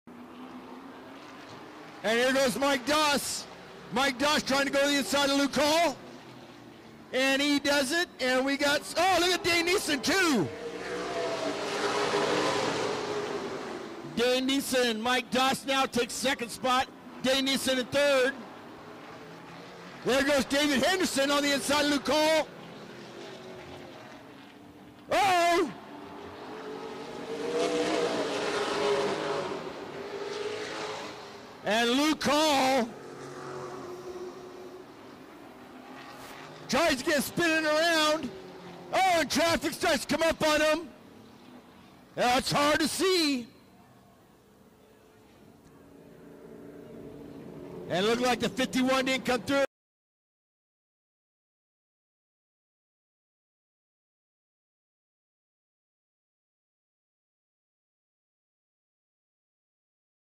Redwood Acres Raceway 7-9-22 Luke Hall And David Henderson Get Together
Luke Hall and David Henderson come together in the late model feature